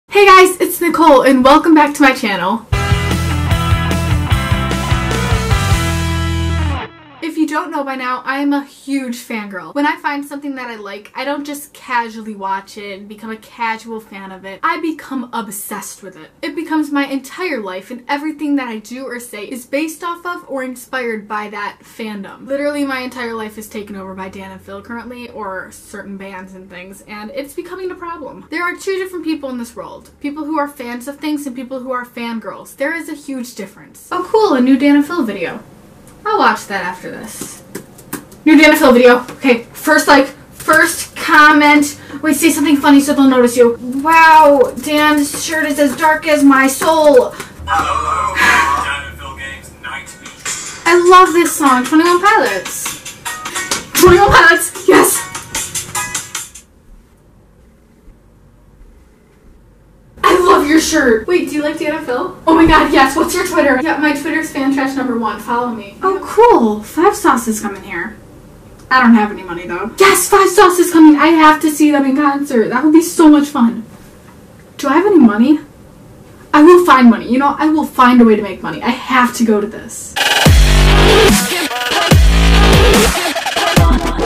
FANS VS FANGIRLS
There is a difference between being a fan of something and being a completely brainwashed and obsessed fangirl like myself.